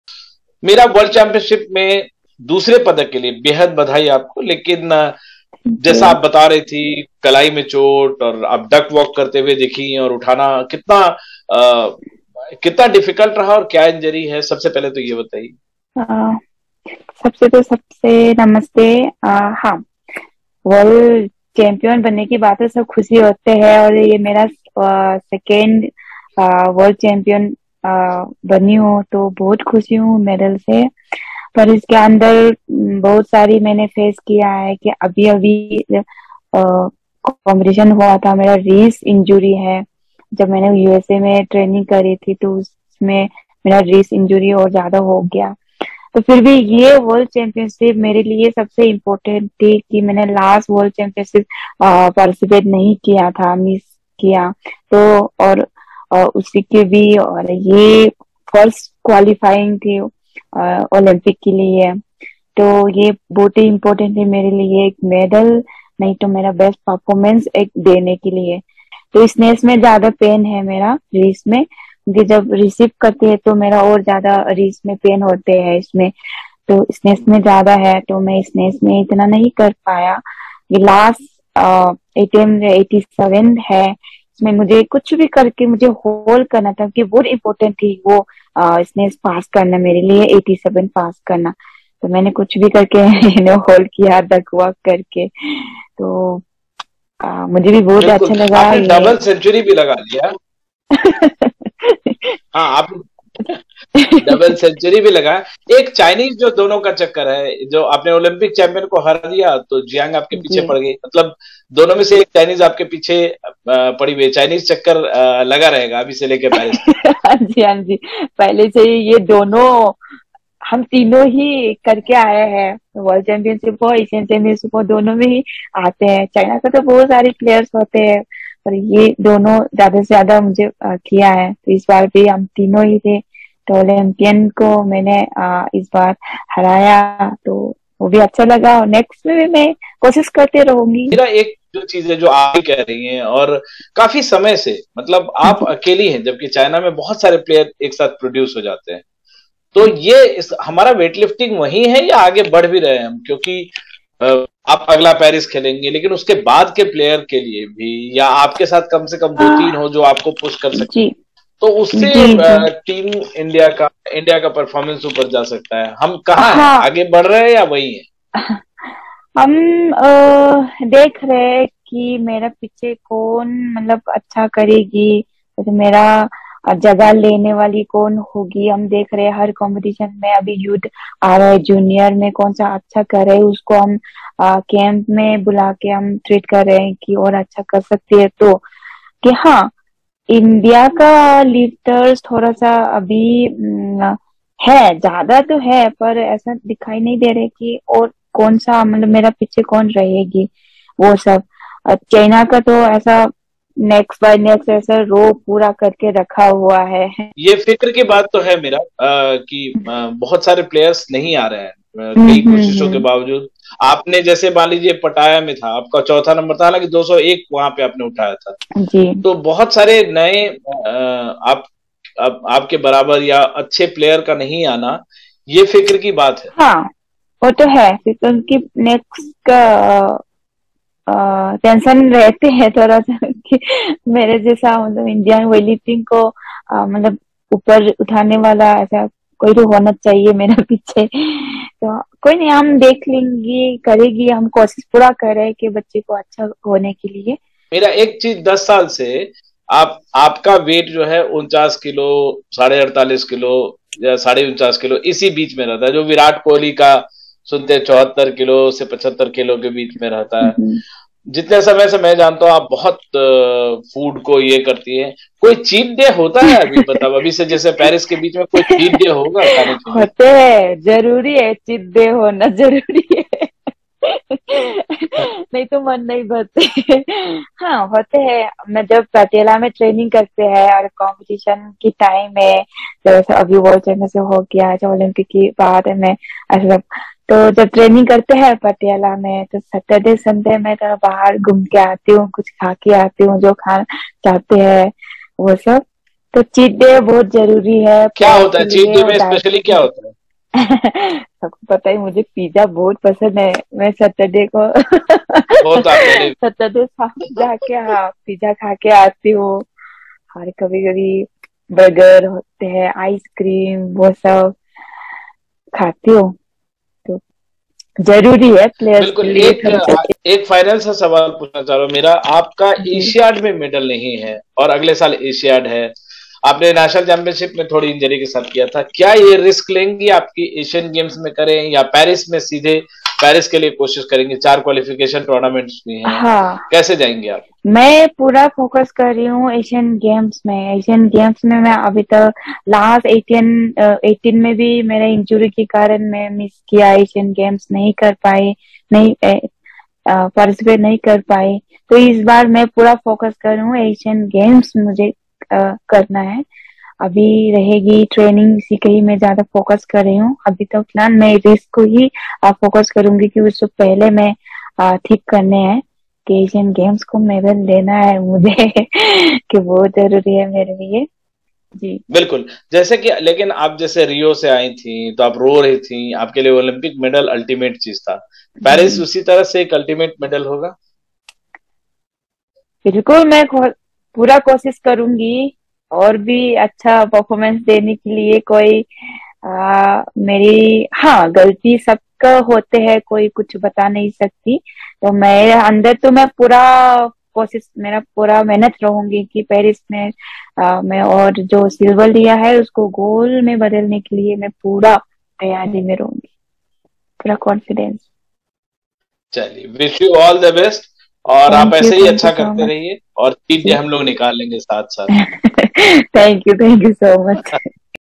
"Happy To Win Silver Medal At World Championships": Mirabai Chanu To NDTV
While speaking to NDTV, India weightlifter Mirabai Chanu said that she was happy after winning a silver medal at the World Championships in Bogota, Colombia. The 28-year-old also said that would look to do better at the upcoming Asian Games, as well as the 2024 Paris Olympics.